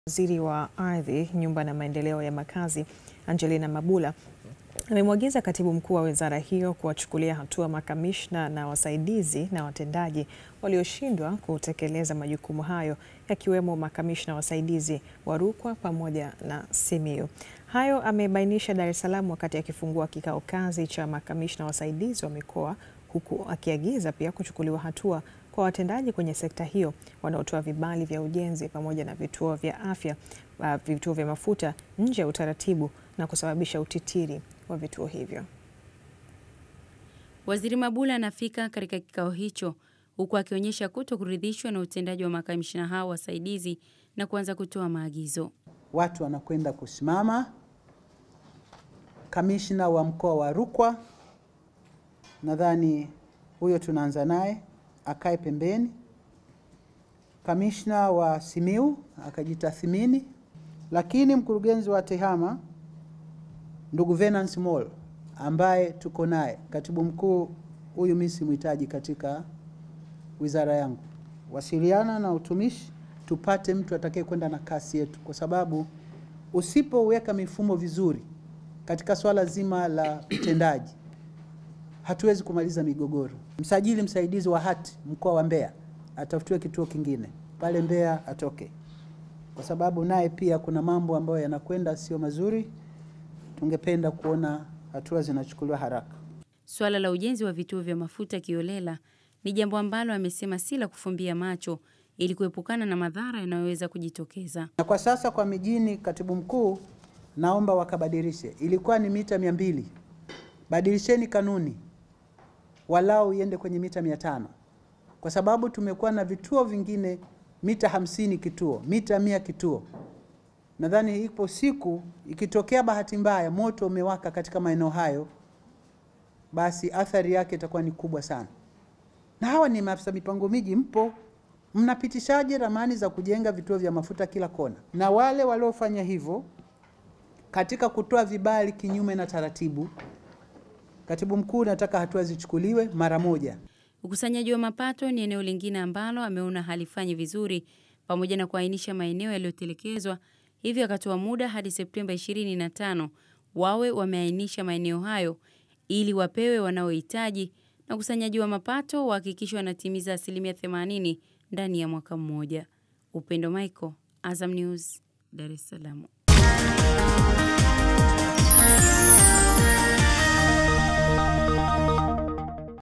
Makamishna ardhi Rukwa na Simiyu wakalia kuti kavu
Waziri wa Ardhi, Nyumba na Maendeleo ya Makazi, Dkt. Angelina Mabula amemwagiza katibu mkuu wa wizara hiyo, kuwachukulia hatua makamisna wasaidizi na watendaji walioshindwa kutekelekeza majukumu yao, wakiwemo makamishna wasaidizi  wa Rukwa na Simiyu.